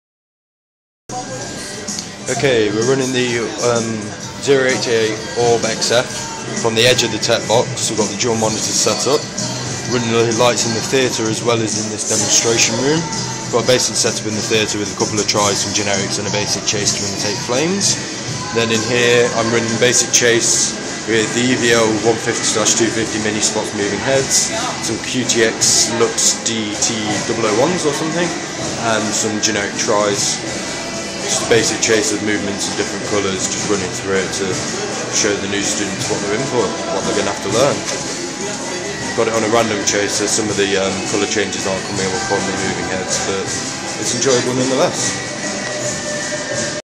Lighting Demonstration
Video evidence of the demonstration work I did during the Gateway Open Day 10/11/12